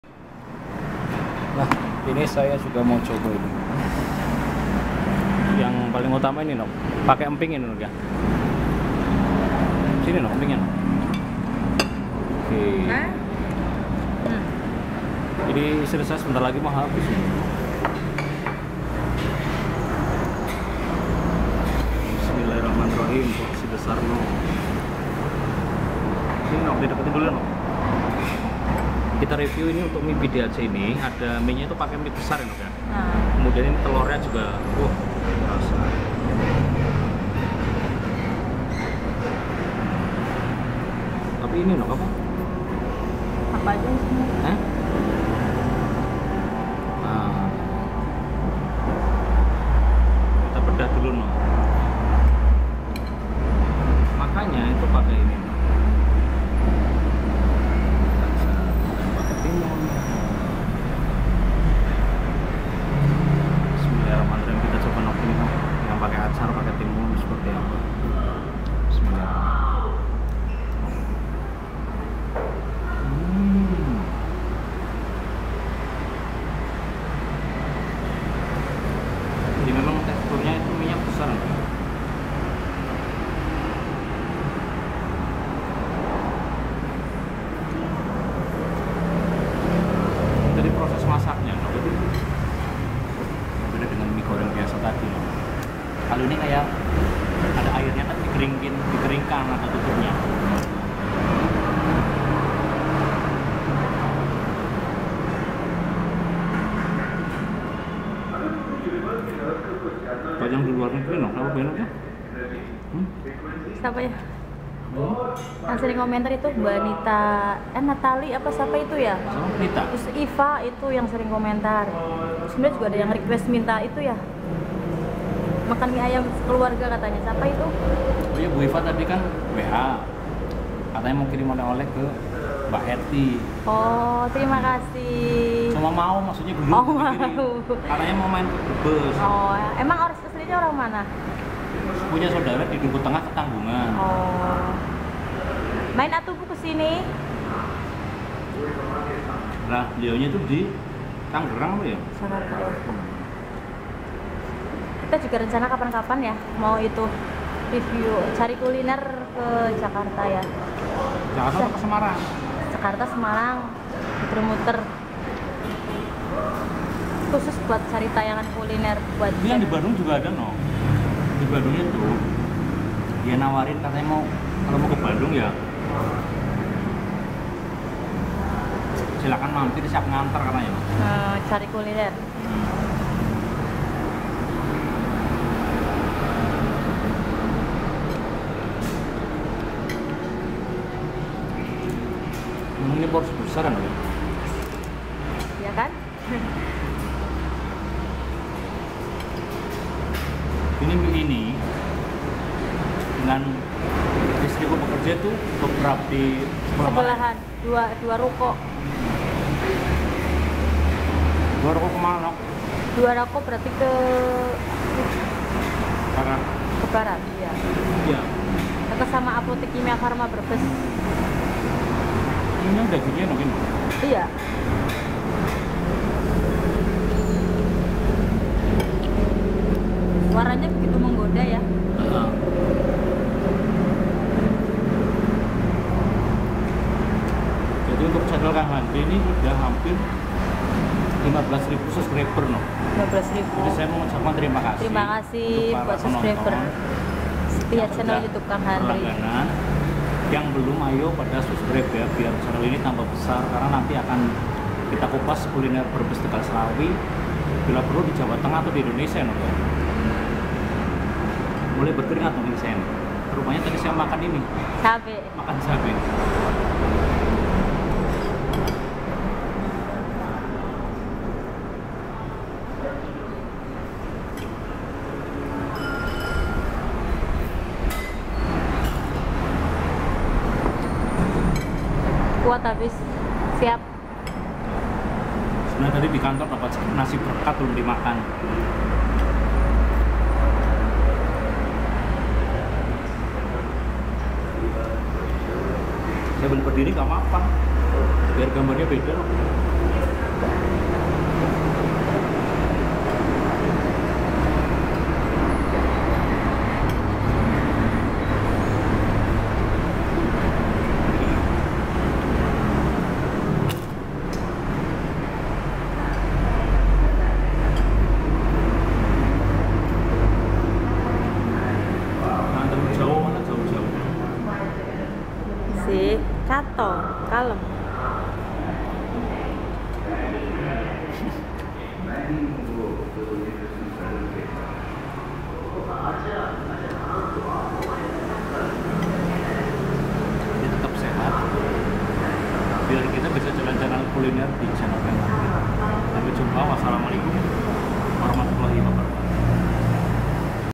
Indonesia Brebes Street Food : Mie Aceh Pidie 2000 Jakarta, Hadir Di Brebes//358//Seri III
Indonesia Brebes Street Food : Mie Aceh Pidie 2000 Jakarta,Hadir Di Brebes Harga dari Mulai 15.000,- Alamat Jl.A.Yani Sebelah Barat Apotik Kimia Farma Brebes //358//Seri III